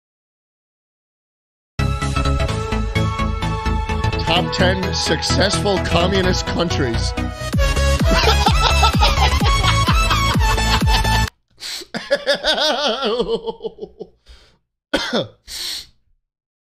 Jschlatt watches a wholesome top 10 video about communism
Jschlatt watches a top 10 video